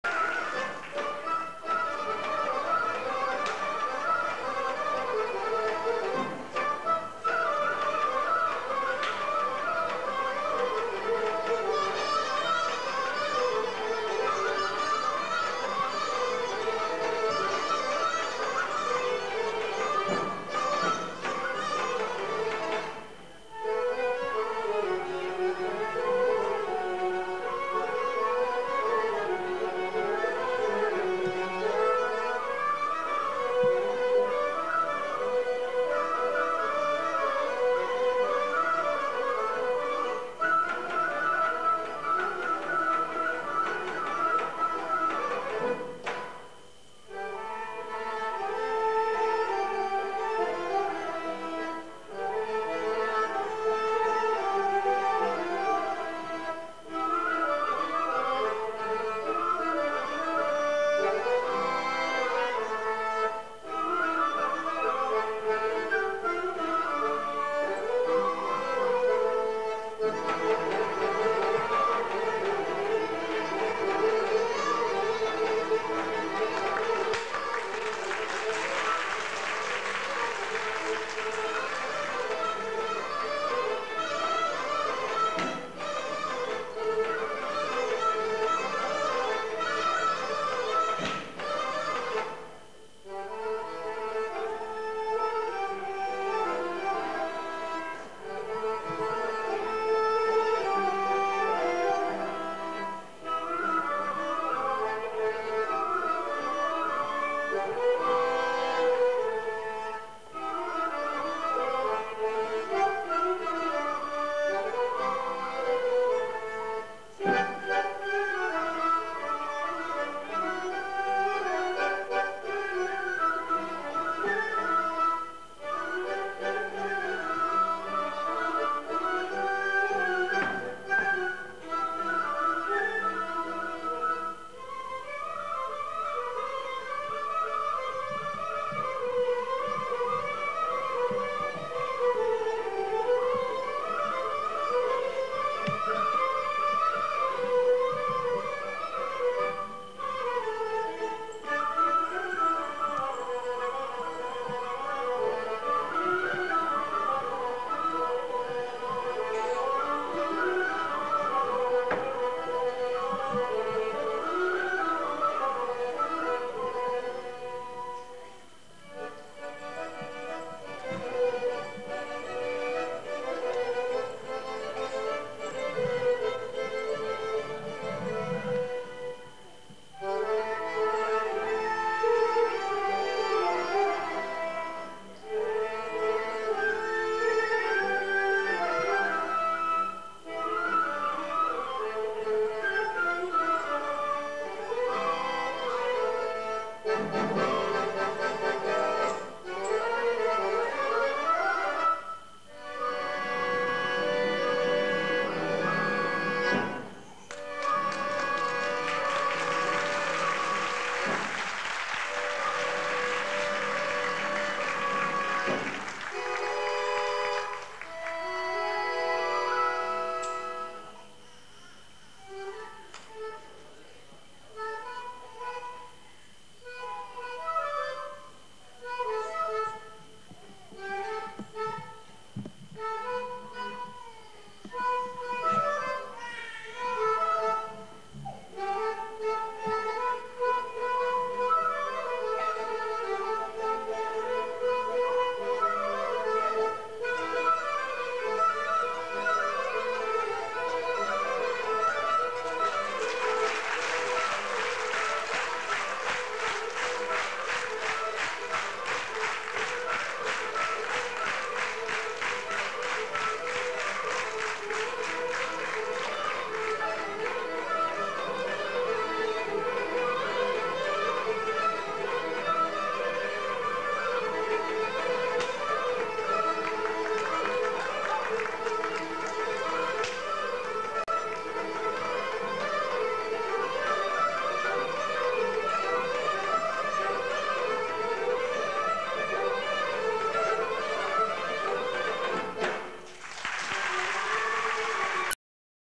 ТАНЦ ЛЮБОВТА НА РАТАЯ . ХОРЕОГРАФИЯ РУМЯН ЗАНЕВ.
АНС, ДОБРУДЖА ГР. СИЛИСТРА. Р-Л СИМЕОН СИМЕОНОВ. СОЛИСТИ МАГДАЛЕНА КЪНЕВА ,РОСЕН АТАНАСОВ, ЕНЧО ЯНКОВ.